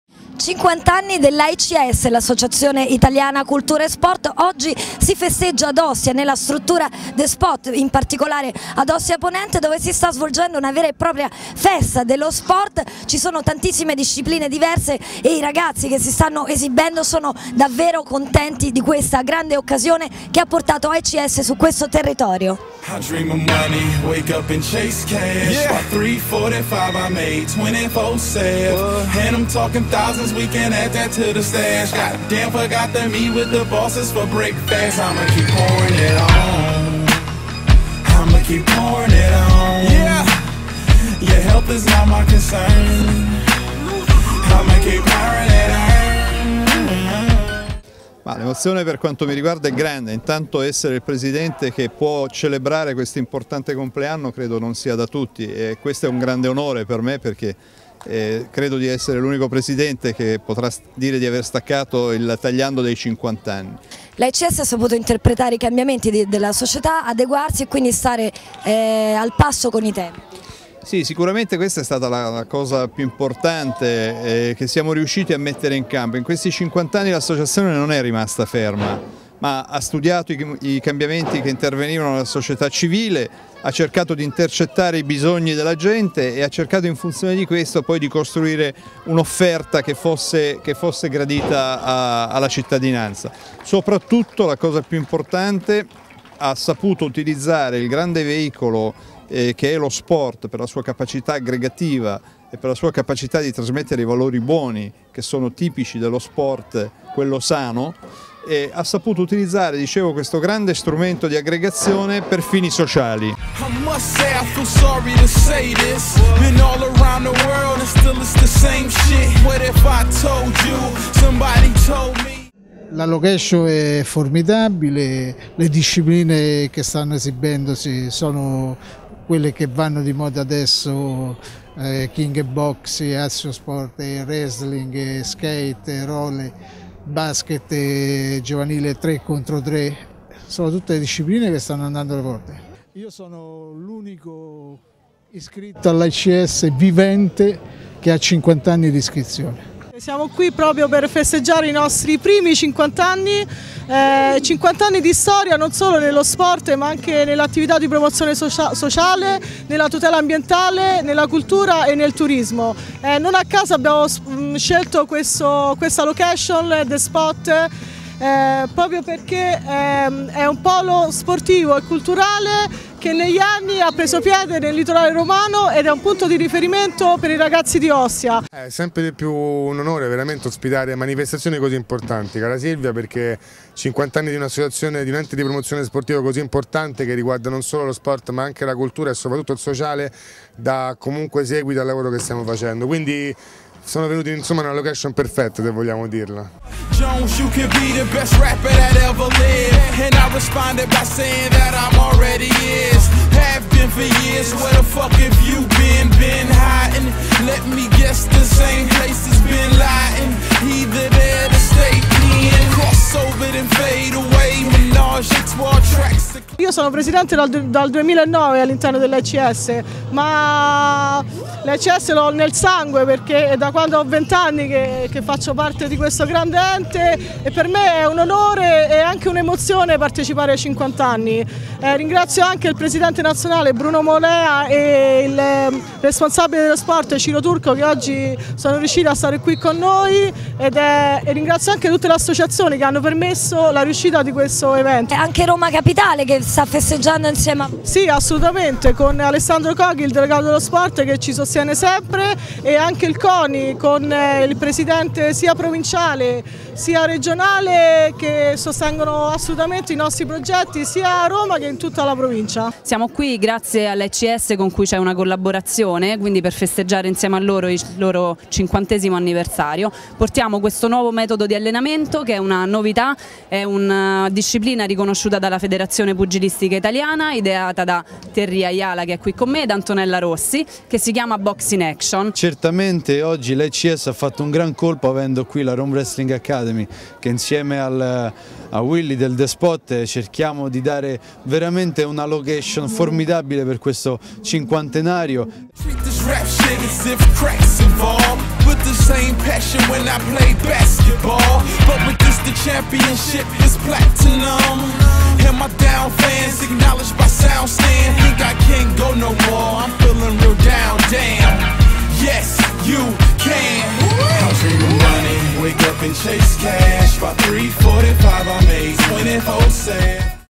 50 anni AICS al The Spot di Ostia
wrestling, skate, roller, basket a 3 e boxe in action, spettacoli di street dance e altro ancora. Queste le discipline sportive e culturali in mostra al The Spot di Ostia in occasione dei 50 anni di attività dell'Associazione Italiana Cultura e Sport.